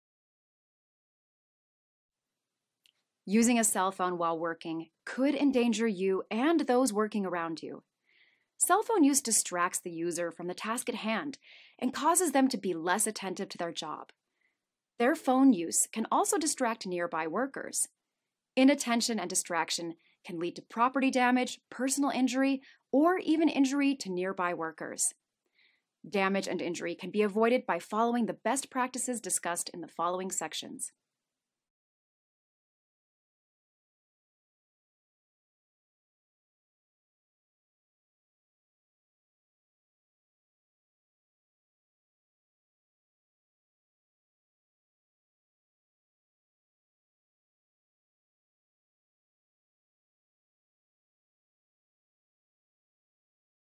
Cell Phone Use in the Workplace
to view the full video and purchase access to our other HR Compliance & Soft Skills courses.

Cell phones have become a standard part of everyday life. They allow us to call, text, find directions, take and share pictures, schedule appointments, deposit money, listen to music, and keep up with social media. While cell phones have many positive aspects, there is a time and place for their use. Using a cell phone improperly at your job site can pose dangers to you and your coworkers. This course covers these dangers as well as best practices associated with cell phone use.